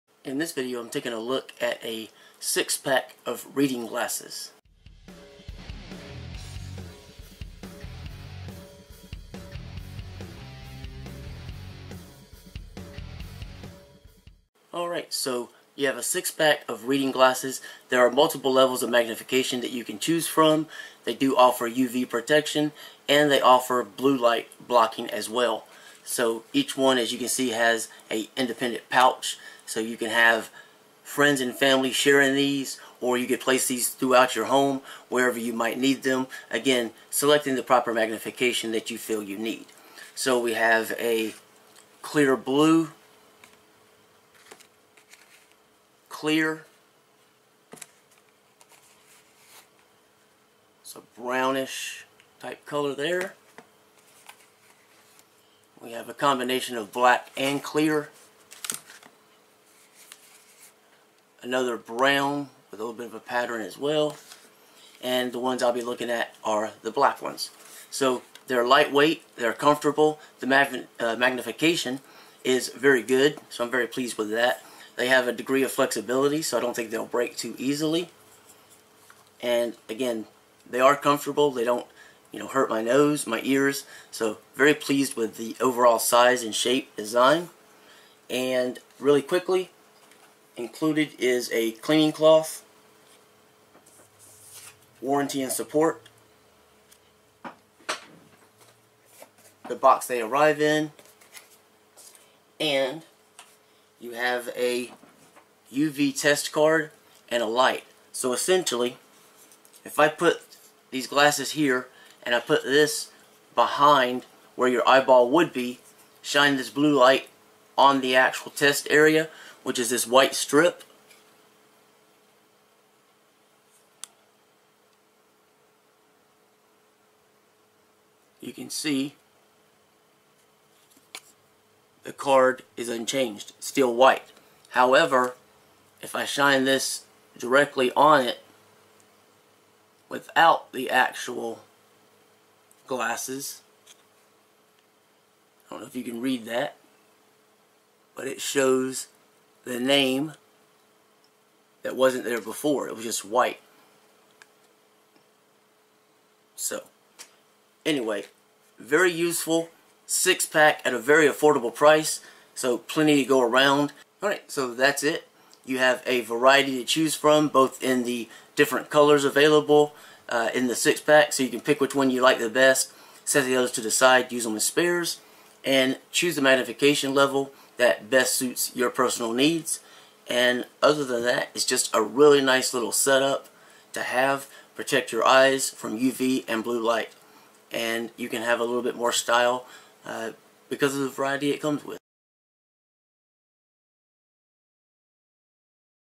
Gaoye 6 Pack Anti UV Glare Blue Light Blocking Reading Glasses
Item(s): Gaoye 6 Pack Reading Glasses Blue Light Blocking for Women Men, Computer Readers Anti UV Glare Eyeglasses Fashion Frames

Review sample provided.